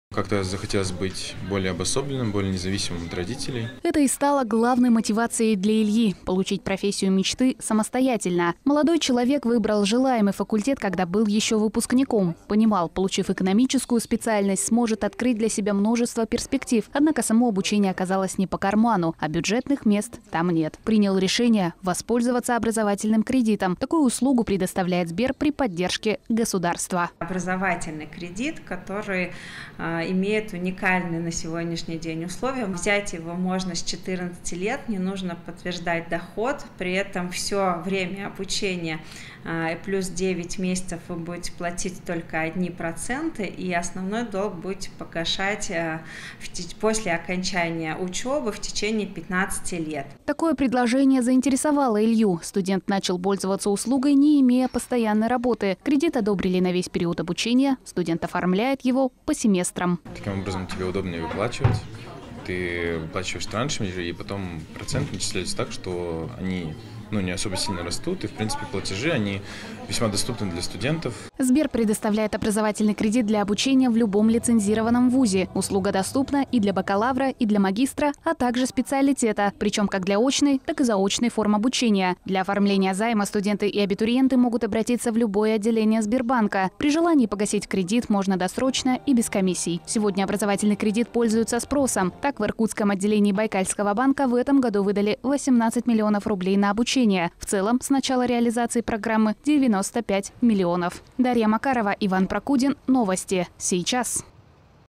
Сделать образование доступным
Профессия мечты может быть доступна. На днях молодой студент рассказал, как самостоятельно и без финансовой поддержки родителей смог поступить в университет. Дорогостоящее обучение парень оплачивает сам. Дарья Макарова узнала, каким способом студенту удалось оперативно найти финансы.